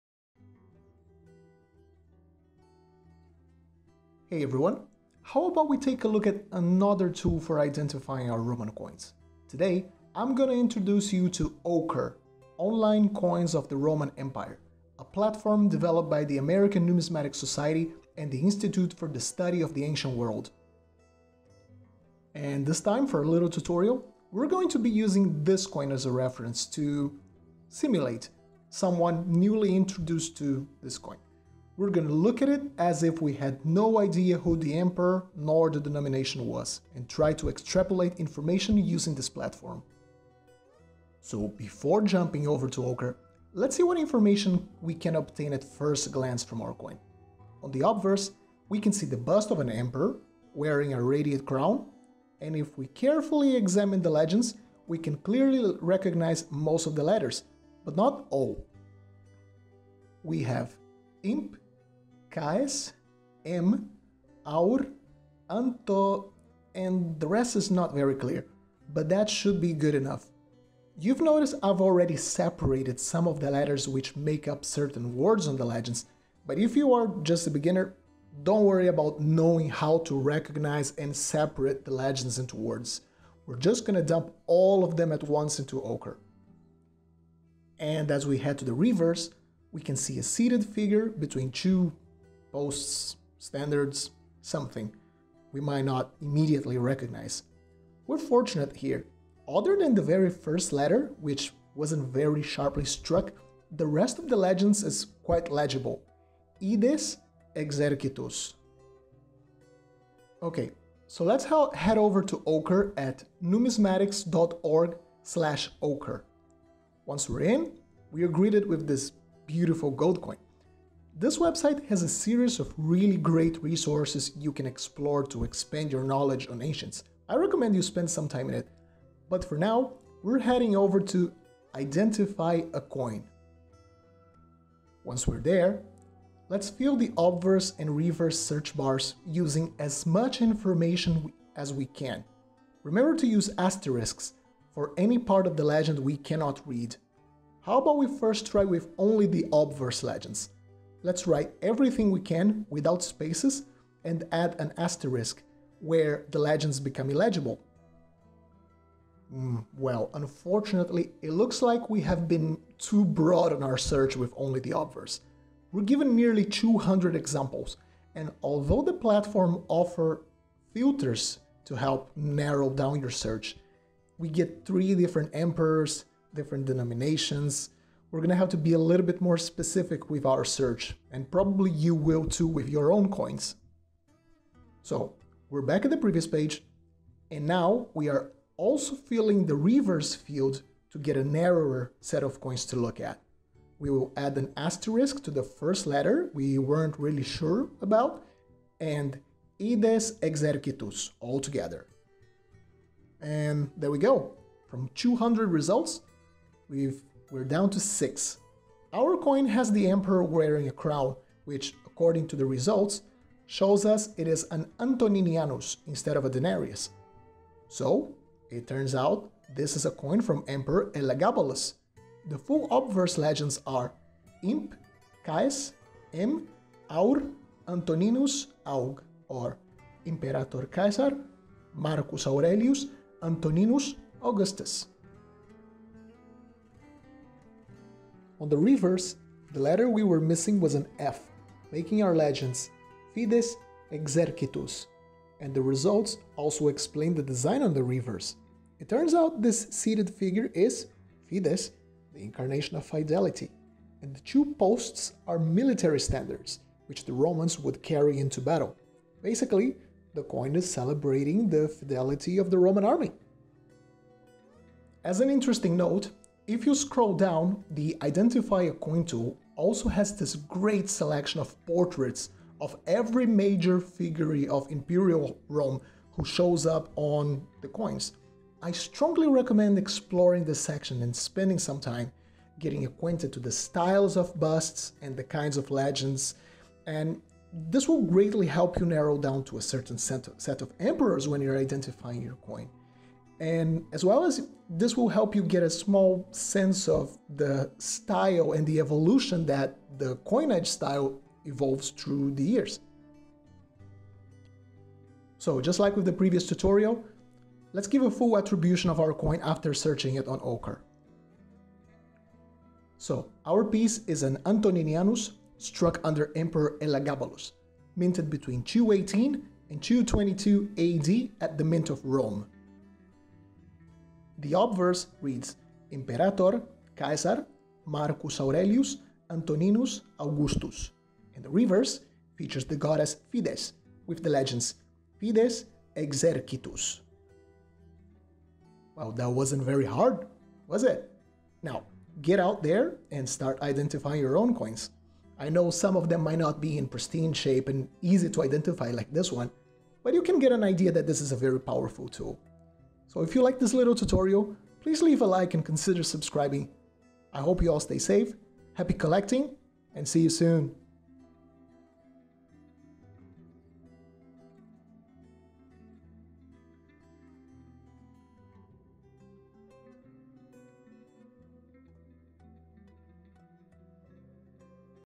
Ancient Coins: Easily identify coins of the Roman Empire
Time for another numismatic tutorial! Today, we are taking a look at OCRE, Online Coins of the Roman Empire, an online tool developed by the American Numismatic Society which I personally use for identifying roman coins.

Their database has a very vast selection of coins, making it easy to find most pieces you should come across easily. Follow me through this video as we fully identify one coin.

All screenshots and footage of the platform is credited to the American Numismatic Society. Thank you for the immense effort made to develop yet another powerful tool for numismatists all around the world to develop their collections, their knowledge, and the field of ancient numismatics as a whole.

Like the drawings on my videos?

Recommended Literature

100 Greatest Ancient Coins
Roman Coins and Their Values
The Handbook of Ancient Greek and Roman Coins
Greek Coins and Their Values